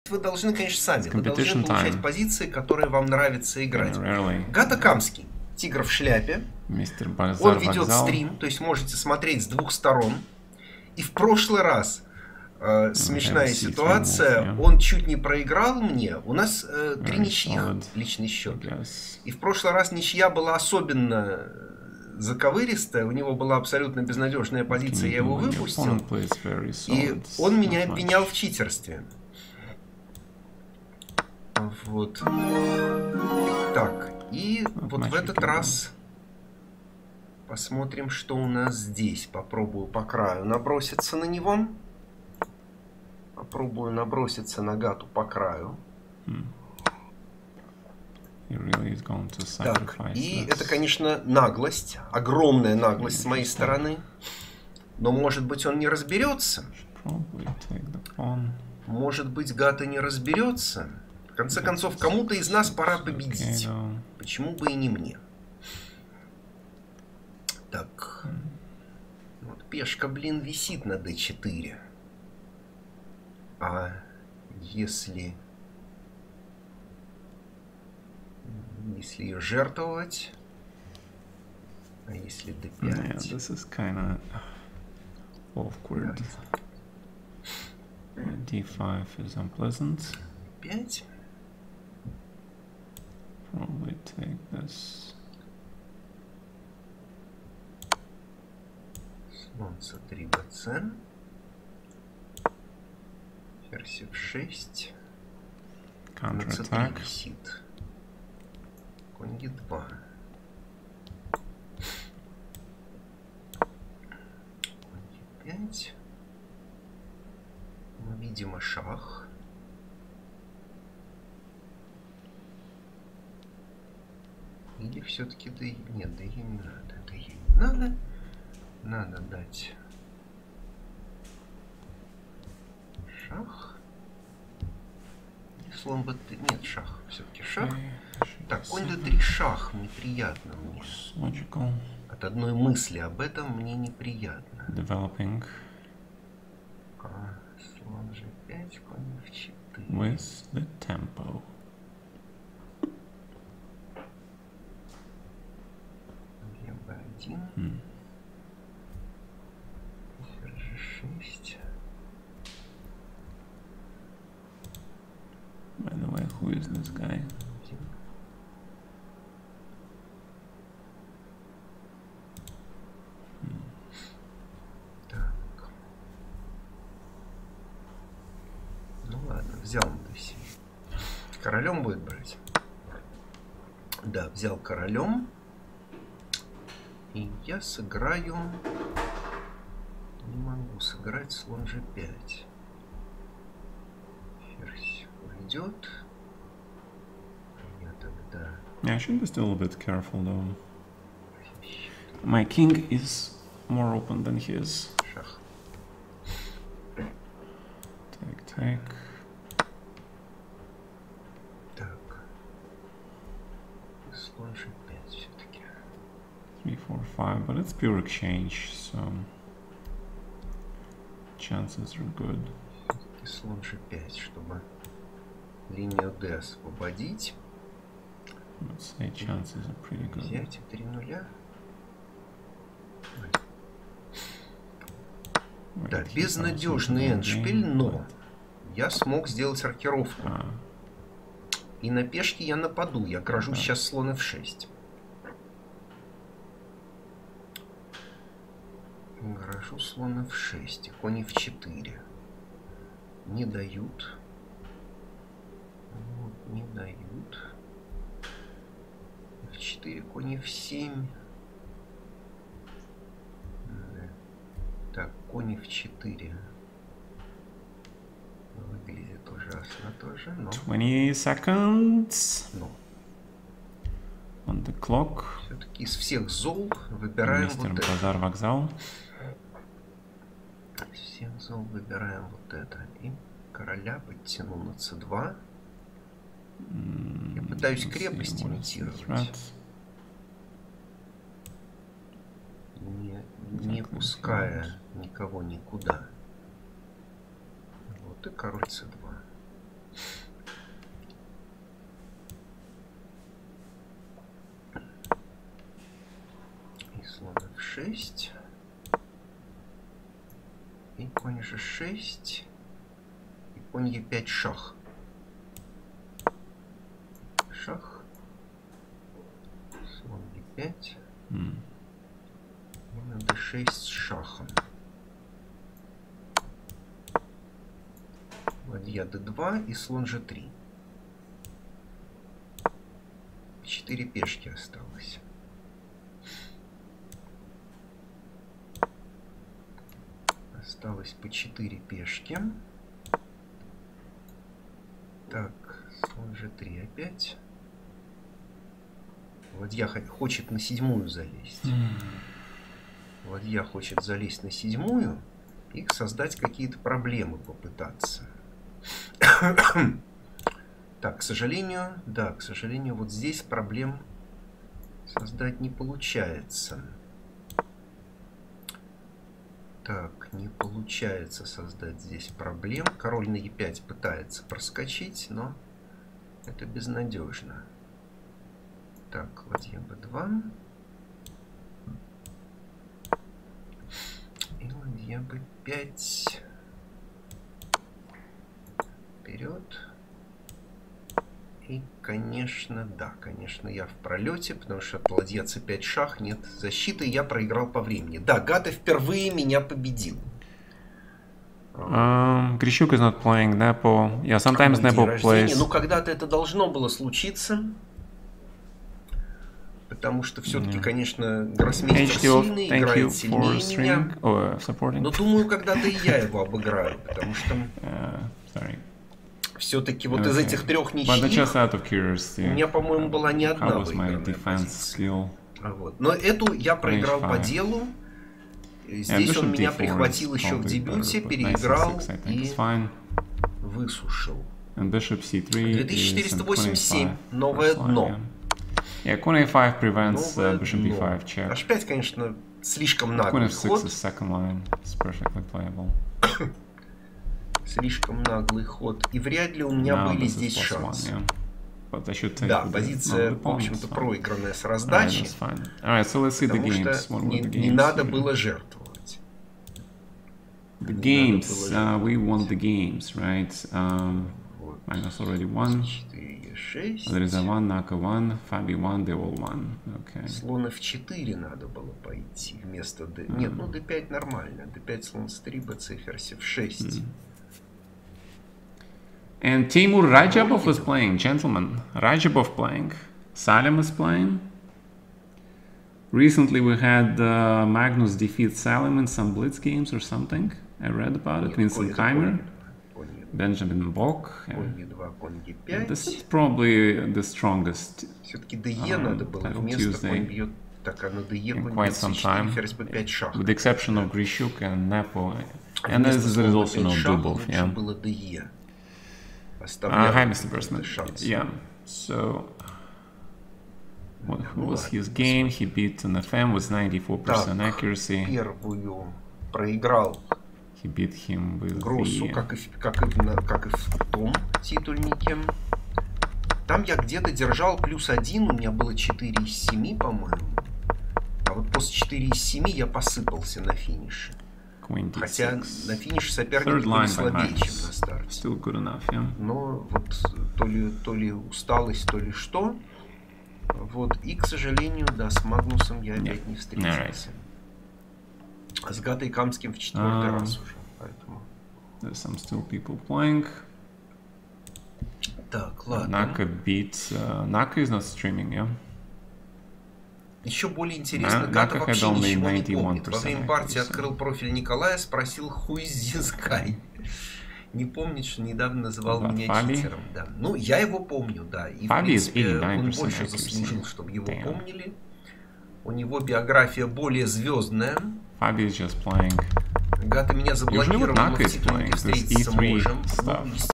ЭТО, КОНЕЧНО, ОГРОМНАЯ НАГЛОСТЬ С МОЕЙ СТОРОНЫ! // IM НИКОЛАЙ ВЛАСОВ vs GM ГАТА КАМСКИЙ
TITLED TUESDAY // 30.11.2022

Гроссмейстер GM Гата Камский
Канал:

bazar-wokzal chess
Международный мастер IM Николай Власов
Канал: